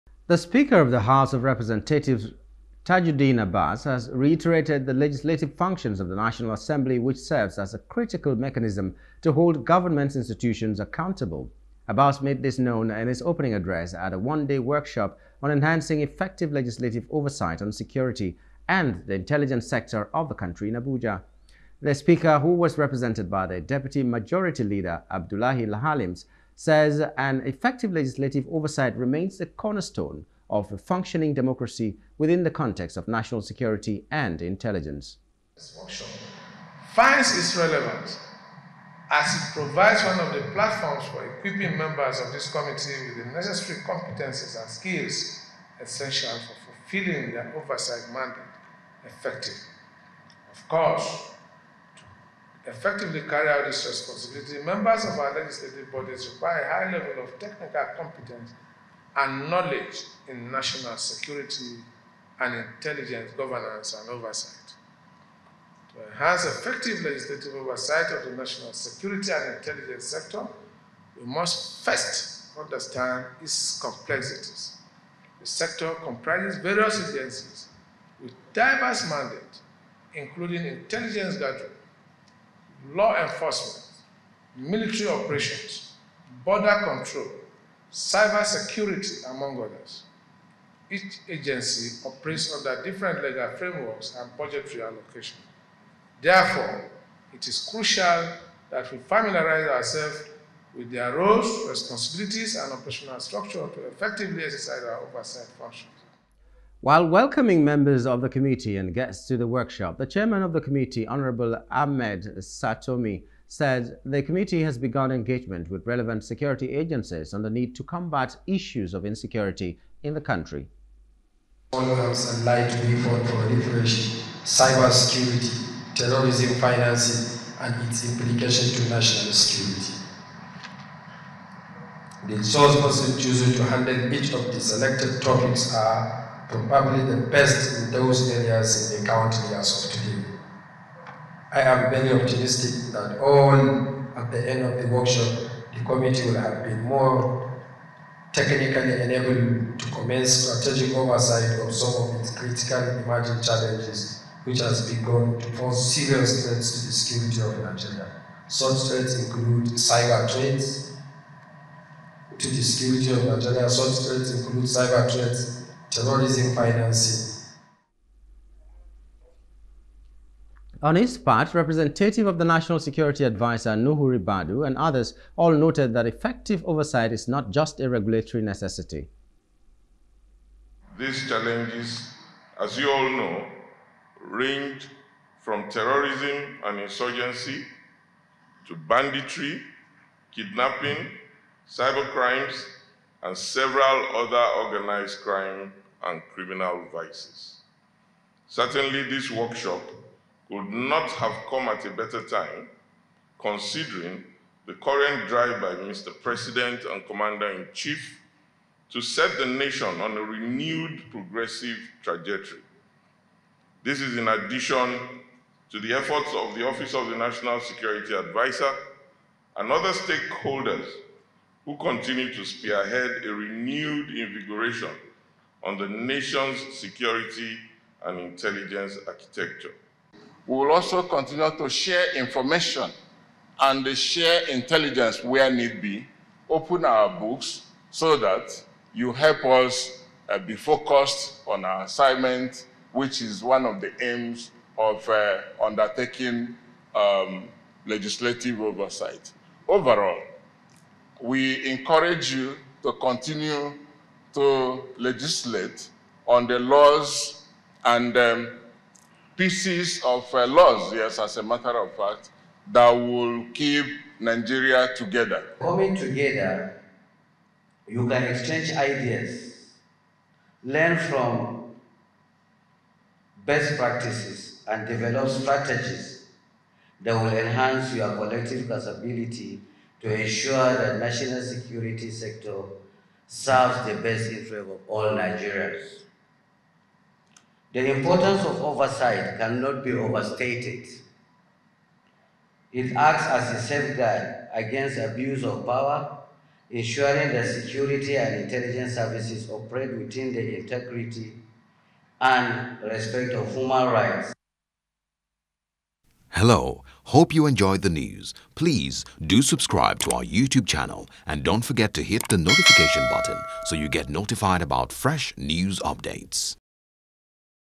National Security Speaker Abbas Harps On Legislative Oversight For Transparency, Accountability
The speaker of the House of Representatives, Rep. Tajudeen Abbas has reiterated the legislative functions of the National Assembly which serves as a critical mechanism to hold government institutions accountable.
He says this mechanism helps to ensure transparency and safeguarding against the abuses of power by government functionaries